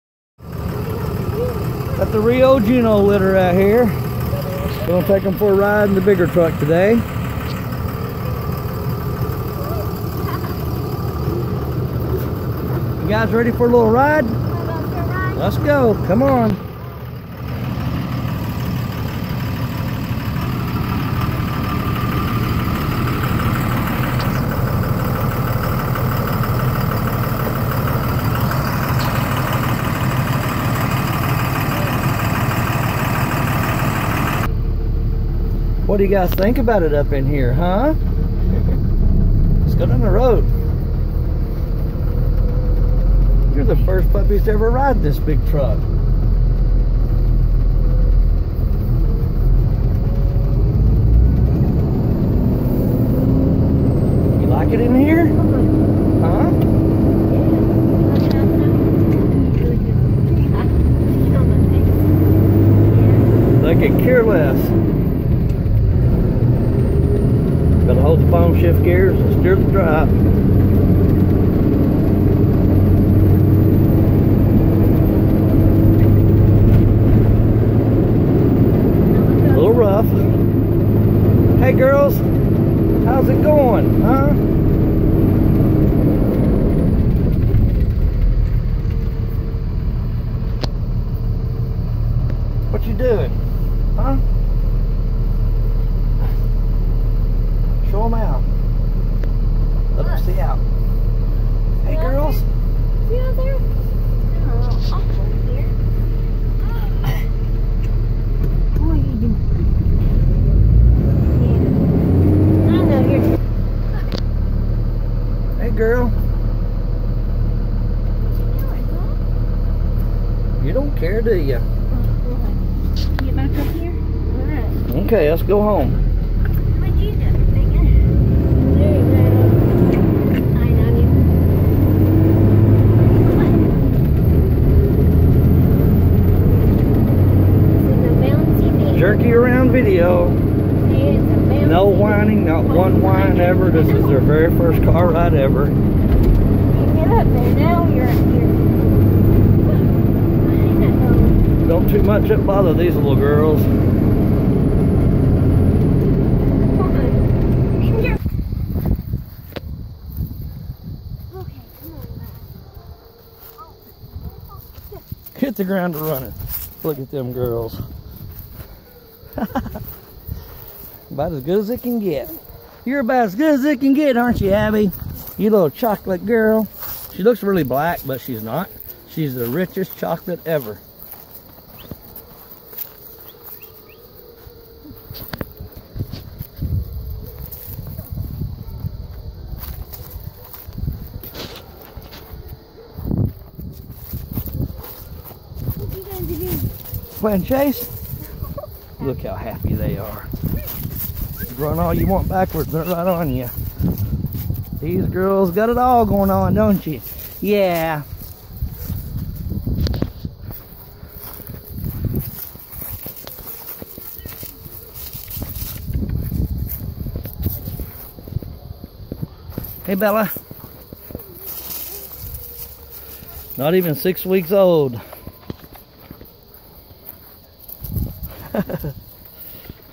German Indian Doodles taking a truck ride
Juno x Rio litter taking a big truck ride for the first time.